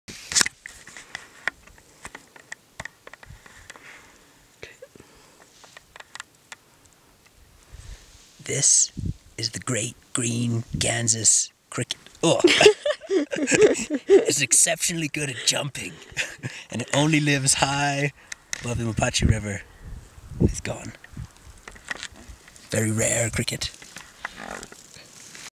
Sir David Attenborough Jr. - Great Green Kansas Jumping Cricket (2012)
Jumping Cricket near the Mapacho River, Peru